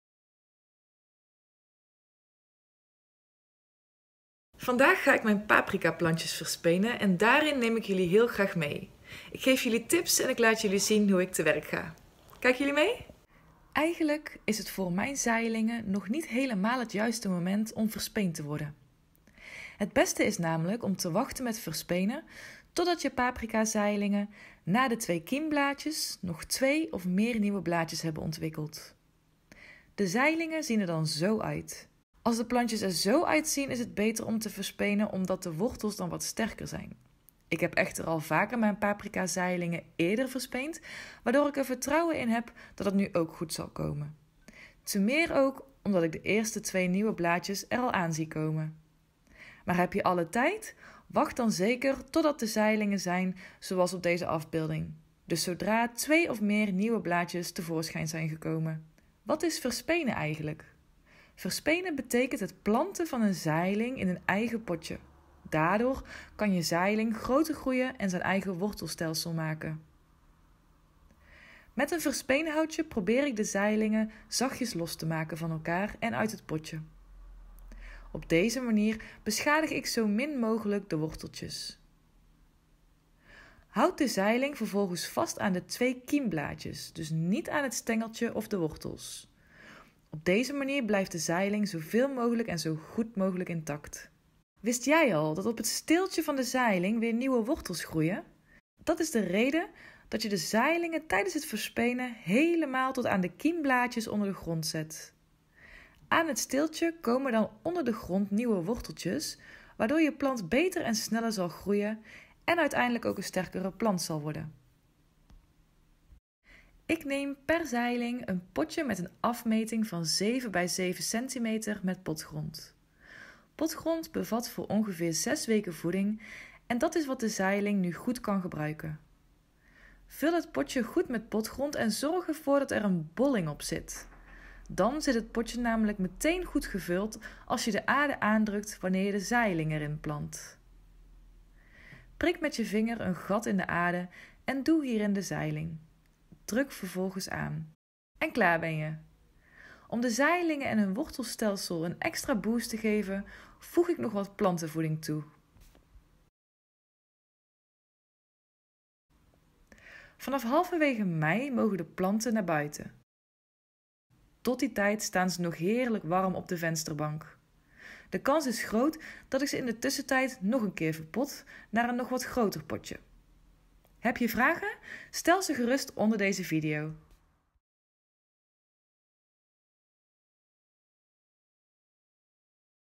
Stadstuinieren - Paprika's verspenen
Het wordt druk op de vensterbank. De zaden zijn allemaal lekker aan het ontkiemen 😍 In deze vlog kun je zien hoe de paprika's die we gezaaid hebben, verspeend worden. Waarom verspenen? Omdat zaaigrond wel goed is om zaden in te laten ontkiemen, maar niet voldoende voeding heeft om de planten te laten groeien. Ook hebben de wortels mee ruimte nodig wanneer je zaailingen gaan groeien.  Meer over zaden en zaden vind je in ons magazine en op onze website.

STADSTUINIEREN Magazine, Het leukste tijdschrift over het kweken van je eigen groente, fruit en kruiden.Voor balkon, dakterras en moestuin.